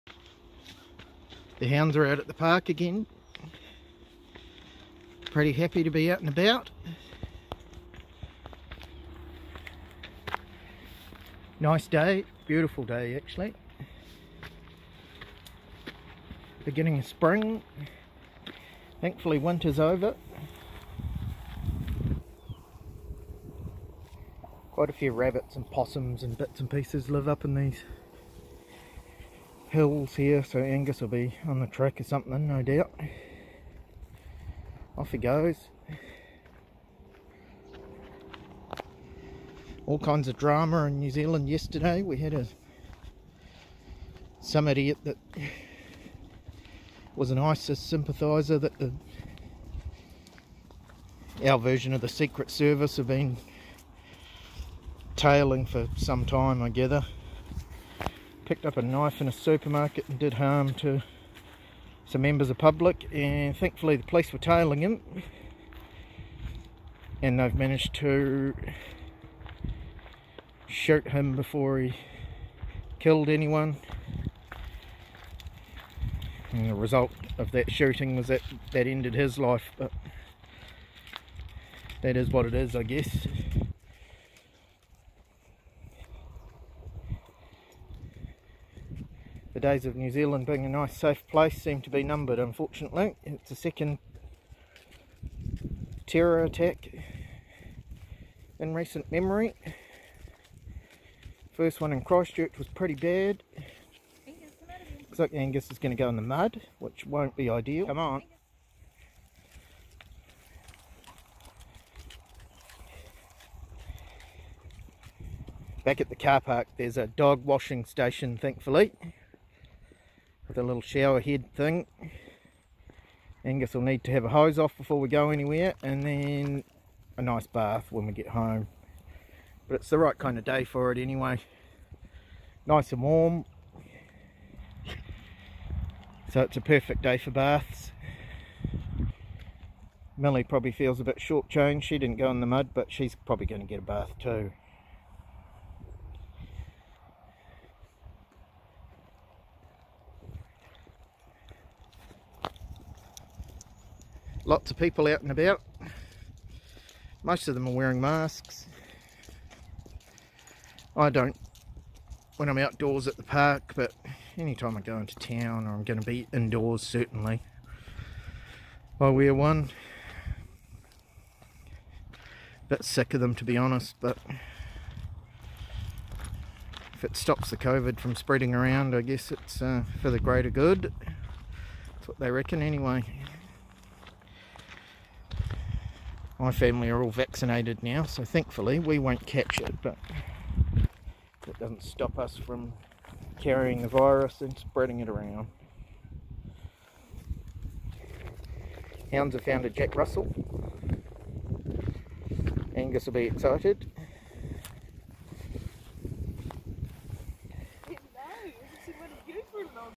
Basset Hounds meet a Jack Russell Terrier
Millie and Angus continue to enjoy the freedom of level 3

Fan Mail

Flat Basset
Taradale
Napier 4141
NEW ZEALAND